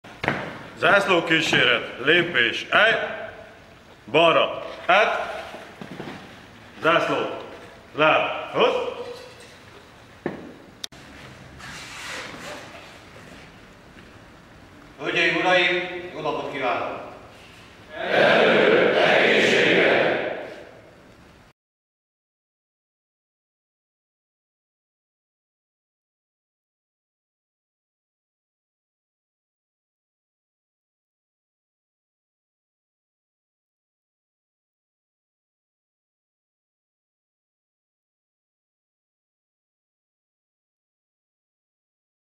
Rendőrnap Hajdú-Bihar Megyében
Elismeréseket vehettek át a Hajdú-Bihar megyei rendőrök.